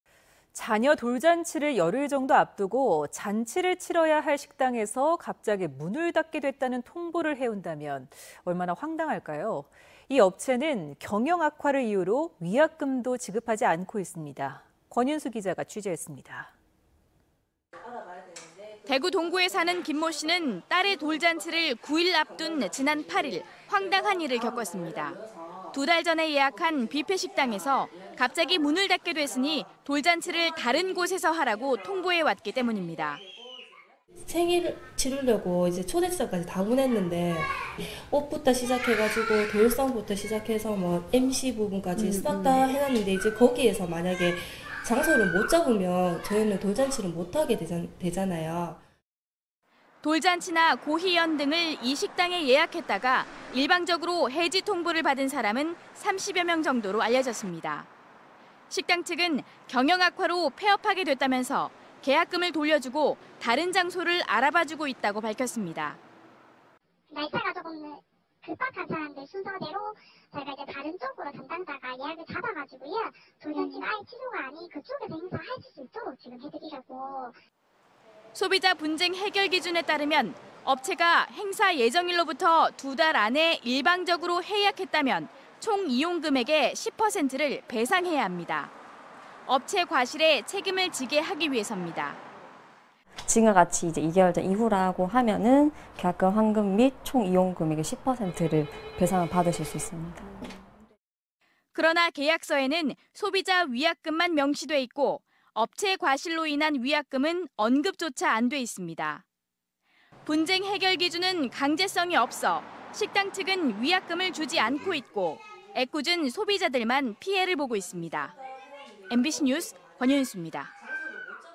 [대구MBC뉴스] 돌잔치 9일 앞두고.. 업체 폐업
내용: 돌잔치 9일 앞두고.. 업체 폐업 - 권윤수 기자